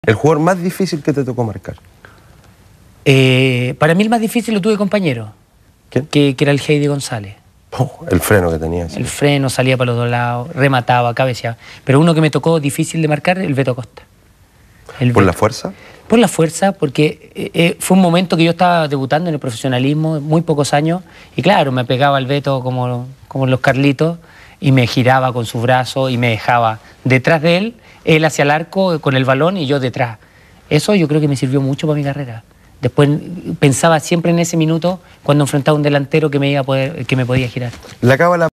Olarra: "El jugador más difícil de marcar lo tuve de compañero"
El ex U. de Chile cuenta quienes son lo jugadores más difíciles que le tocó marcar en su época como futbolista.